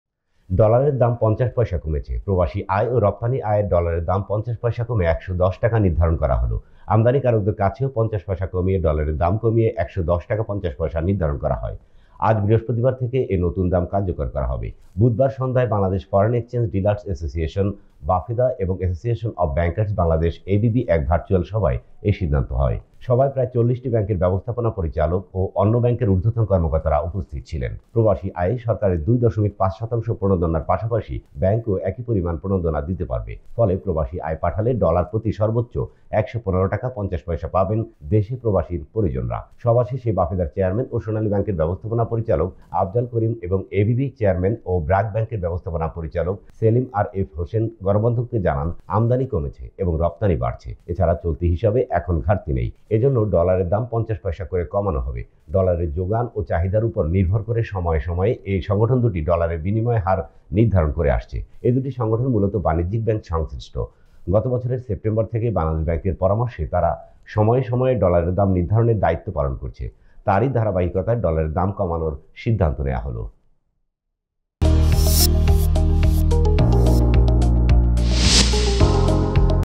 রেমিট্যান্স-রফতানি আয় নগদায়নে কমলো ডলারের দাম | দেশবন্ধু টিভি | DeshbandhuTV
▶🔶 Search Me in google : deshbandhu TV